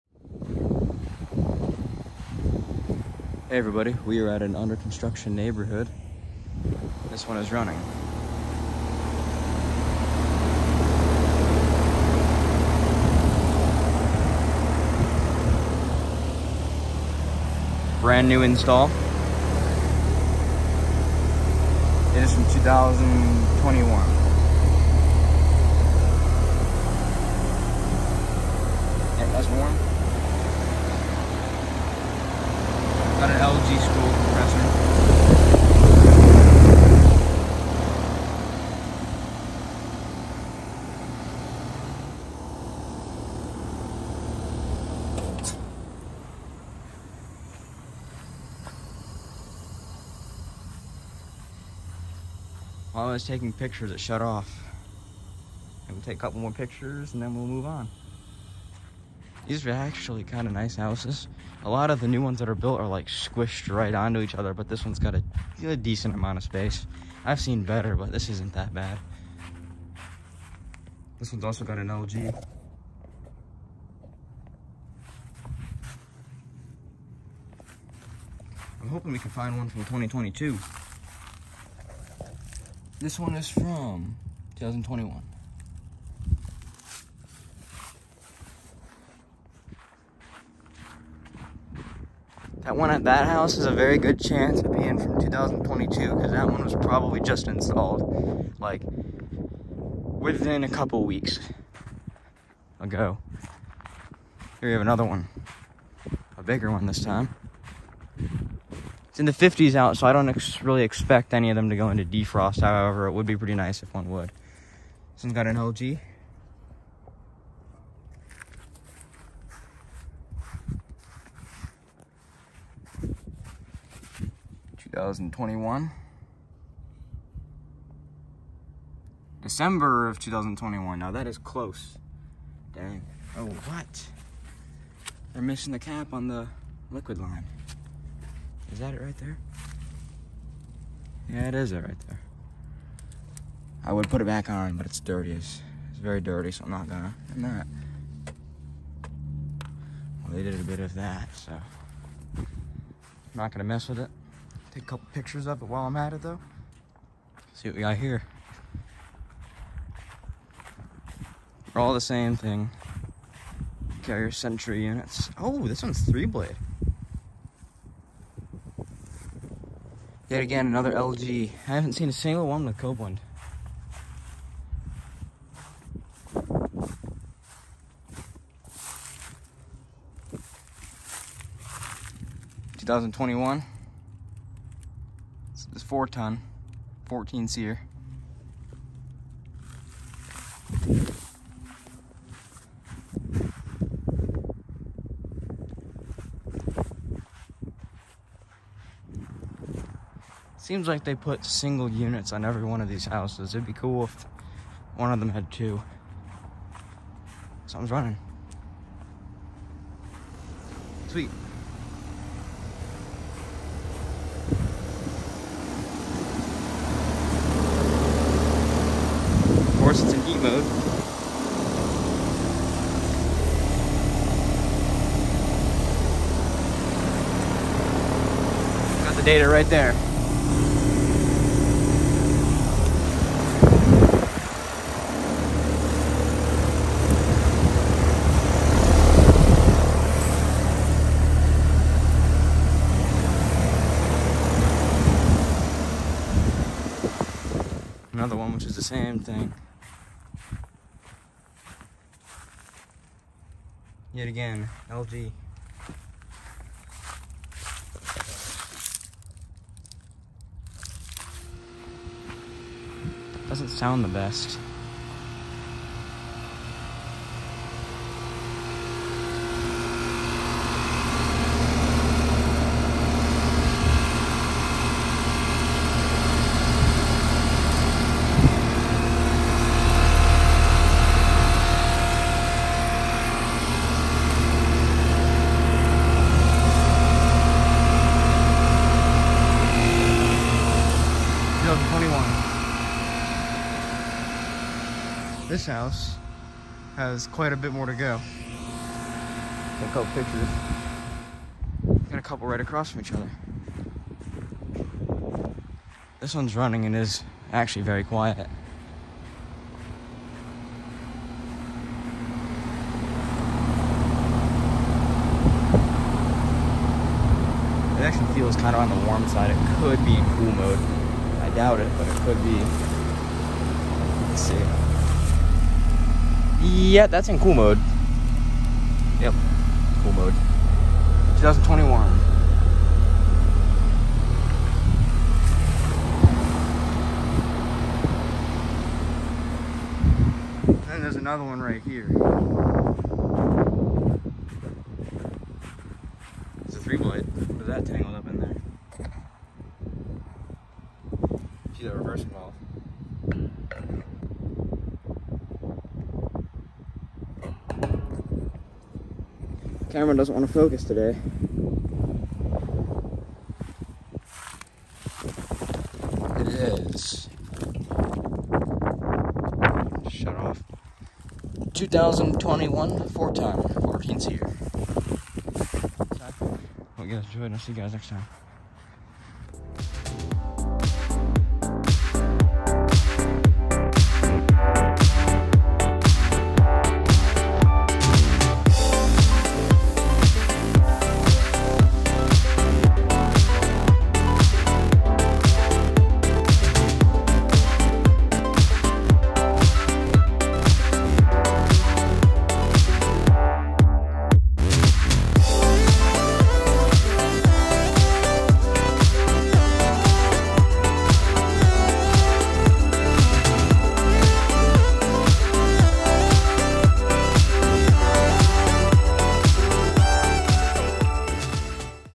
Lots of New Carrier Sentry Heat Pumps - New Construction HVAC Heaven
Several new Carrier Sentry Heat Pumps at a new construction neighborhood. I also found a unit from 2022 but I decided it belonged in It's own separate video.